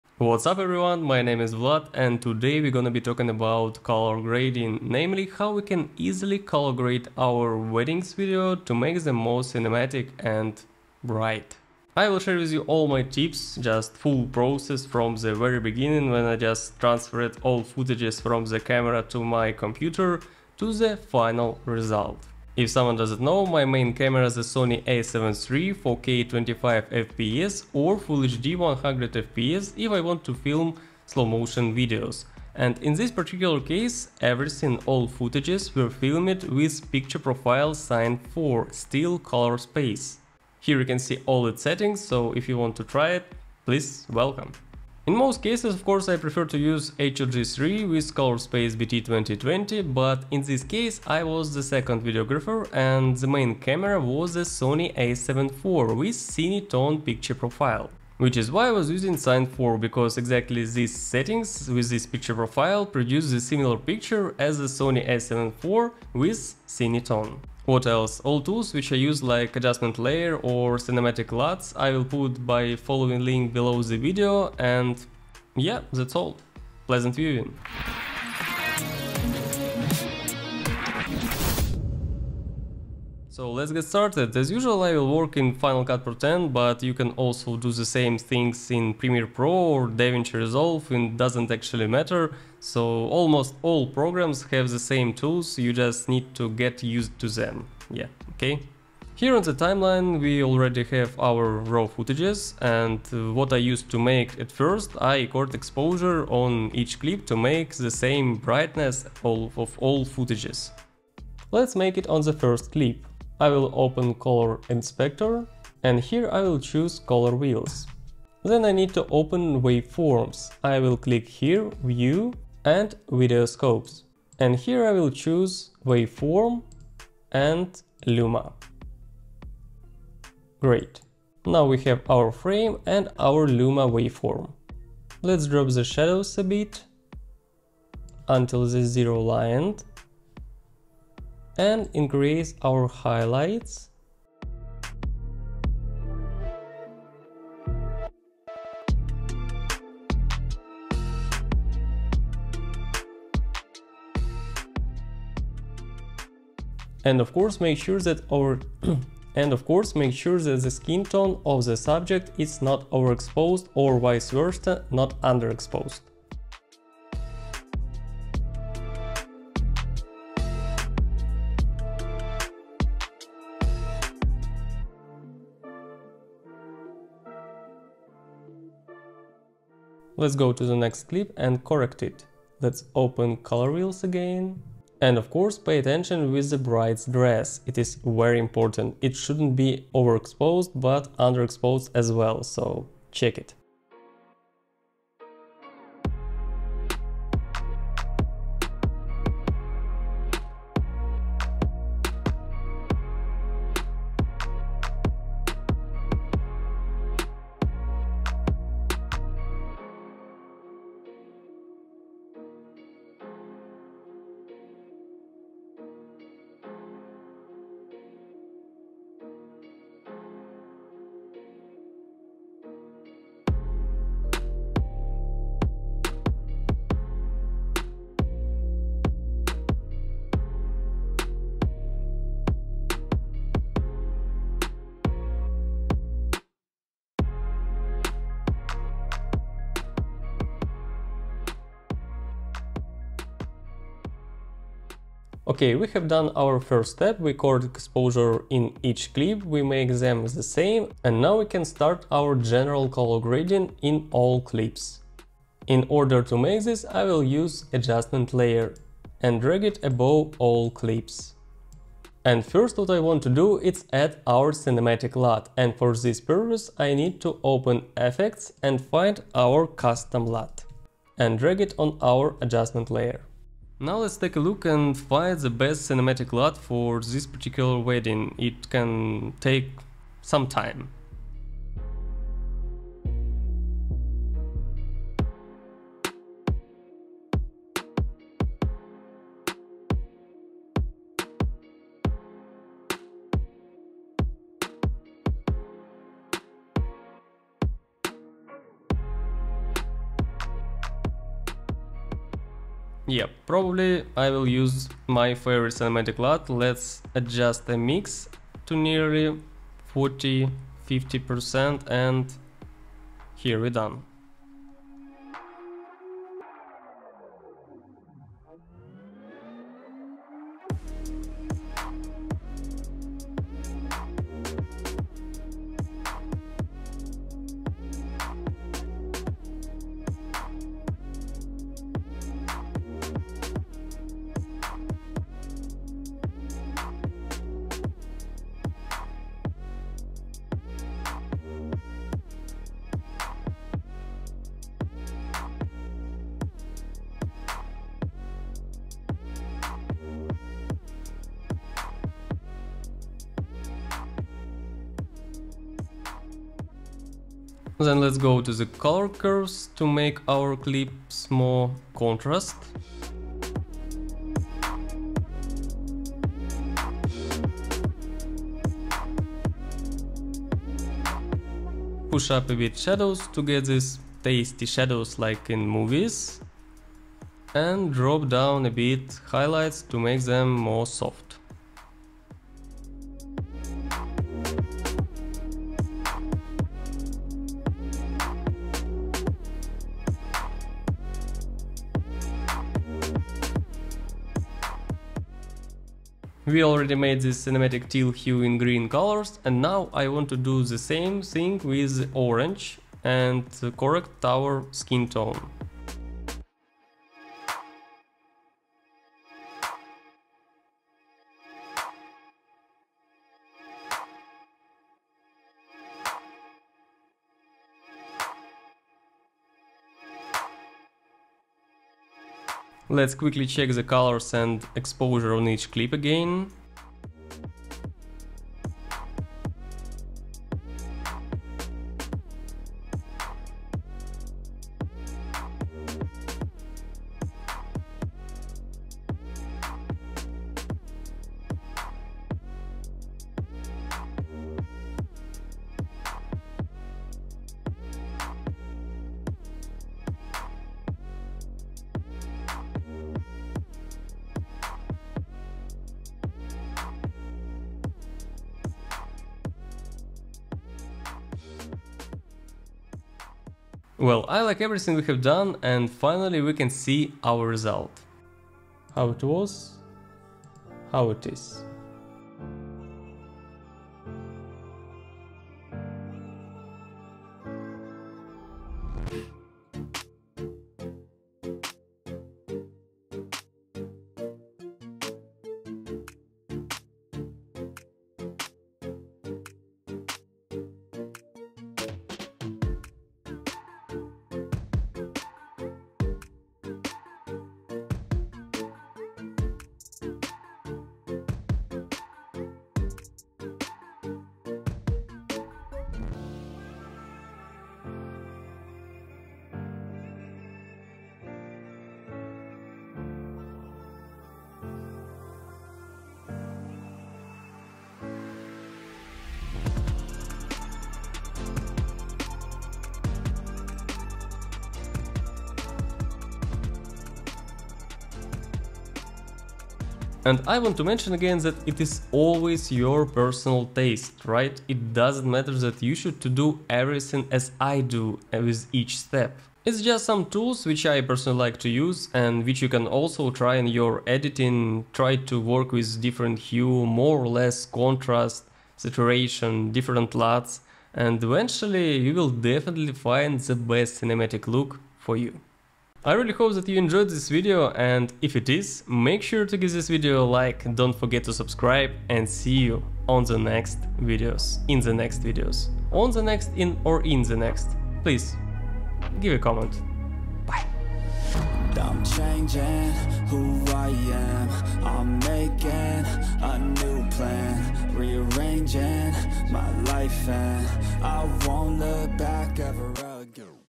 How to Color Grade Wedding Video: Make Cinematic Look in Final Cut Pro X
How to Color Grade Wedding Video? Cinematic Look in Final Cut Pro X
What’s up everyone, my name is Vlad and today we gonna be talking about Color Grading, namely how to esialy color grade your wedding videos and make them more cinematic and bright.
0:00 - Introduction
0:25 - My camera and picture profile
1:17 - Correct exposure through Color Wheels
4:00 - Add Cinematic Lut with Custom Lut
5:35 - Work with the Hue/Saturation Curves
5:58 - Work with the Color Curves
6:35 - Correct Skin tone
7:02 - Correct Skin tone
8:50 - Cinematic Color Grading